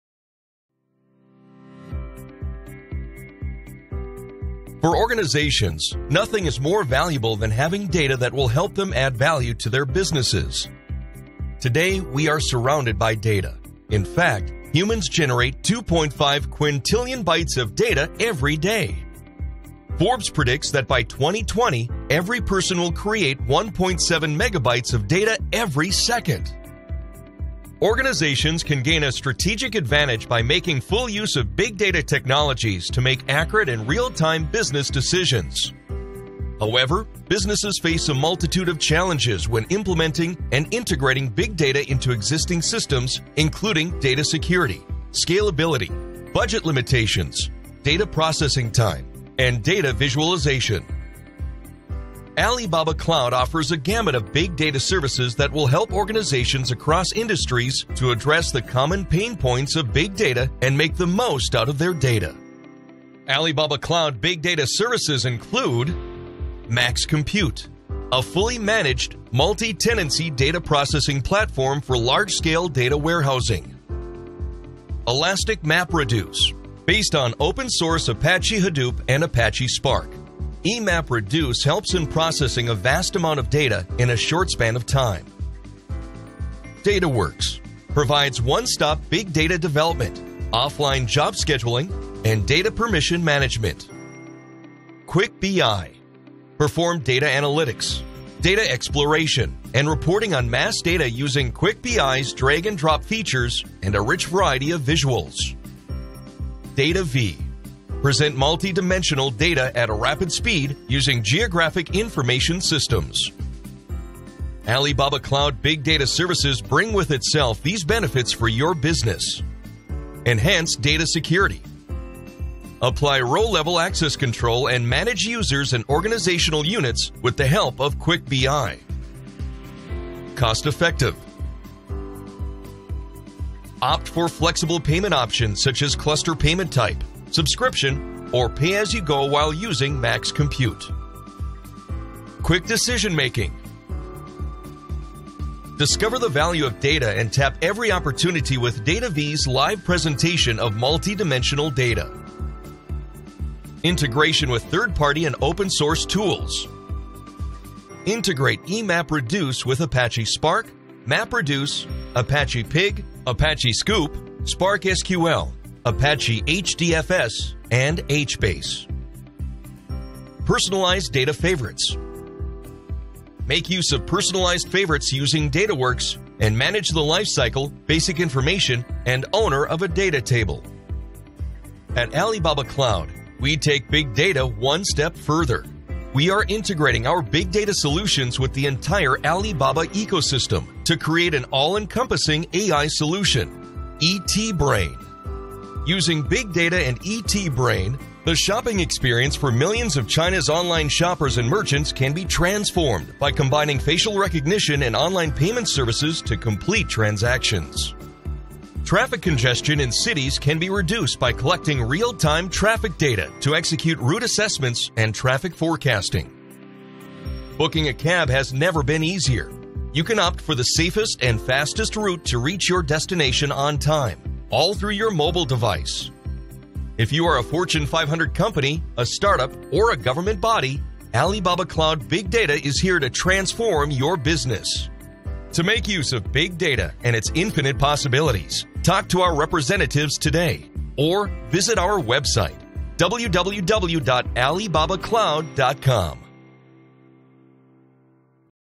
Get Valuable Insights for Your Businesses with Alibaba Big Data Solutions
Simplify Big Data processing and application development with Alibaba Cloud Big Data solutions.

From data warehousing to Big Data modeling, get valuable insights from your data with Alibaba Cloud DTPlus suite of products.